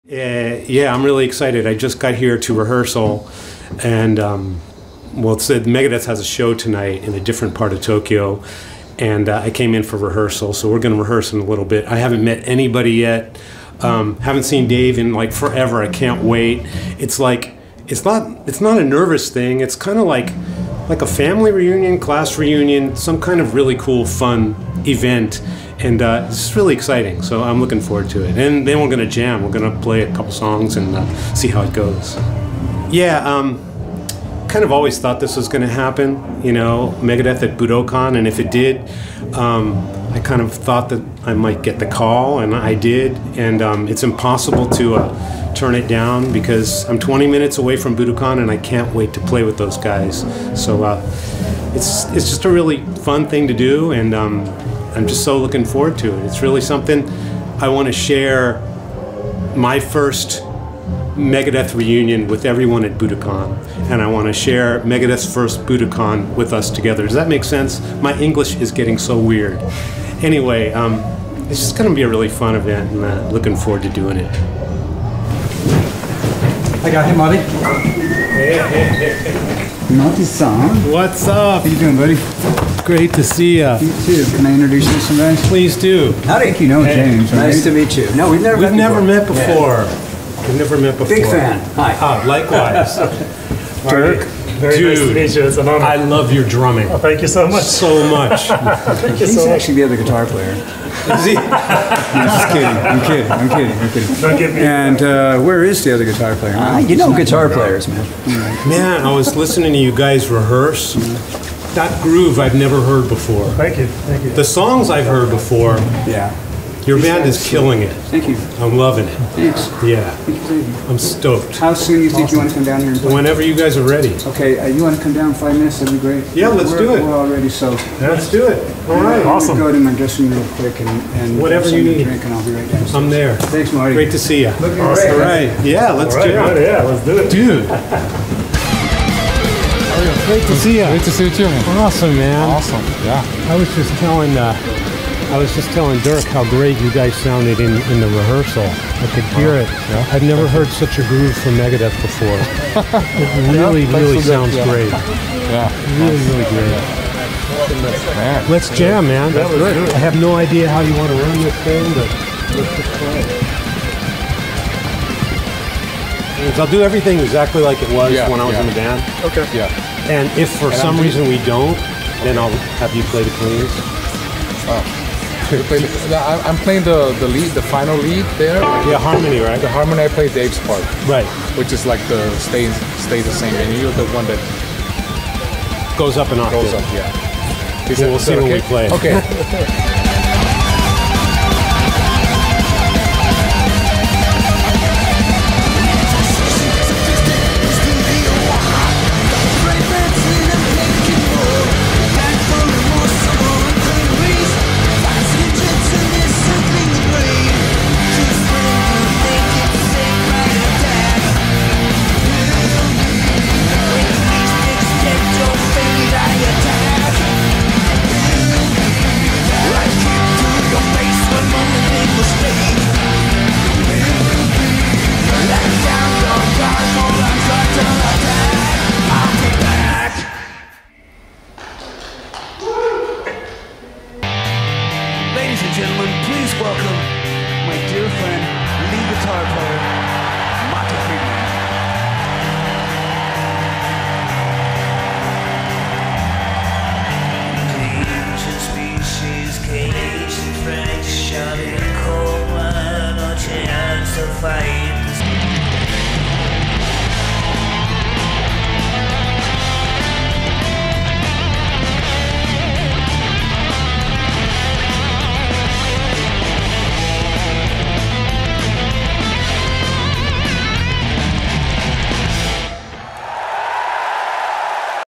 MEGADETH: Budokan Preshow. MARTY FRIEDMAN reunites with the band for the first time in 23 years!
The previous full video got claimed, but in case people still wanna see the reunion, I cut it up to some highlights. This is a momentous occasion that must be celebrated, please let it stay up!

Im not even a monetized channel so Im not making anything off of this lol